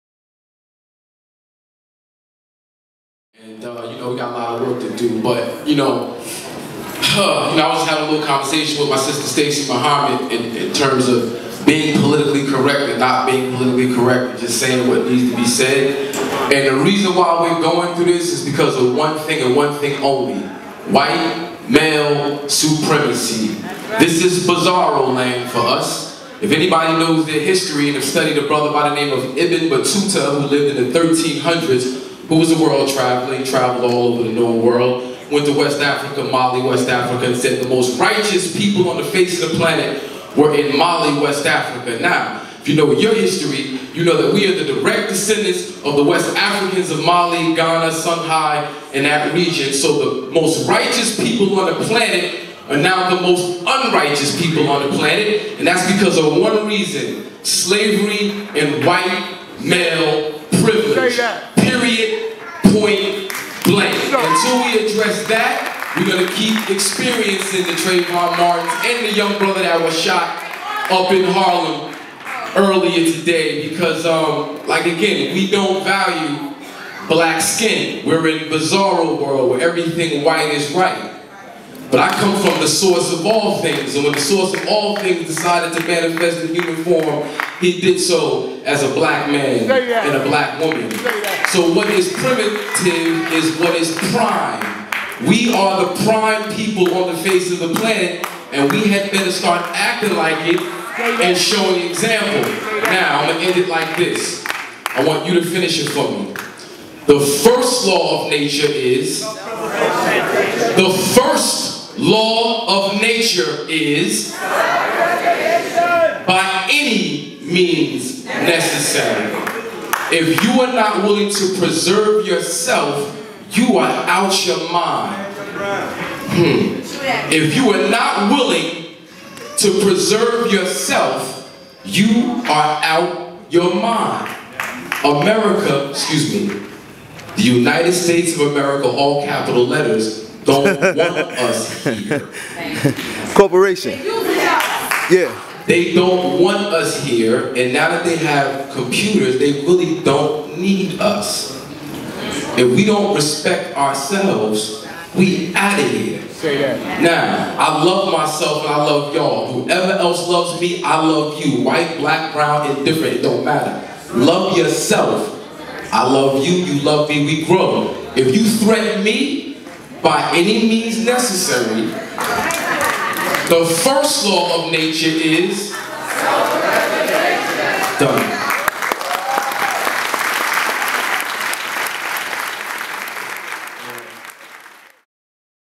Hakim Green speak on the Trayvon Martin situation.
illflix.com behind the scenes at the Jasiri X video shoot for his tribute to Trayvon Martin.